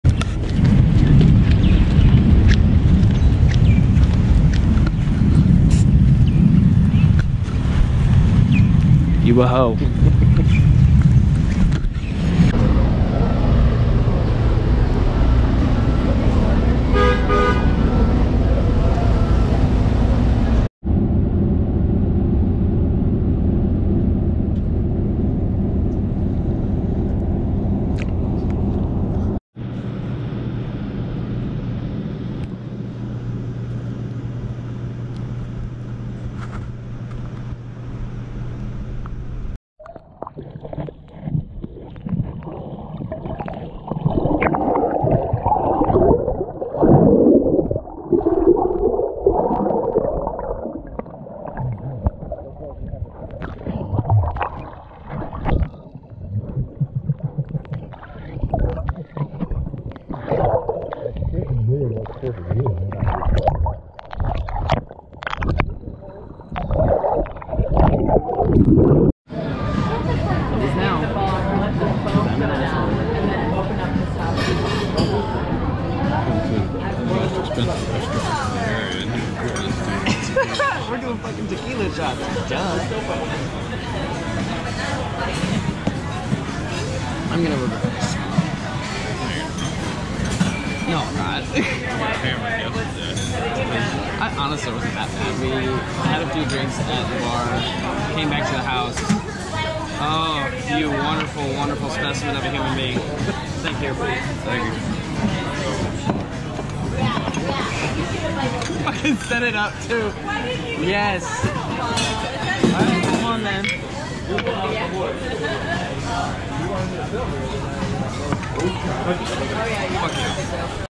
Hawaii 2020: Goofing Off
Oahu, Hawaii: January 2020. Oh, just a short compilation of goofing off and alcohol consumption...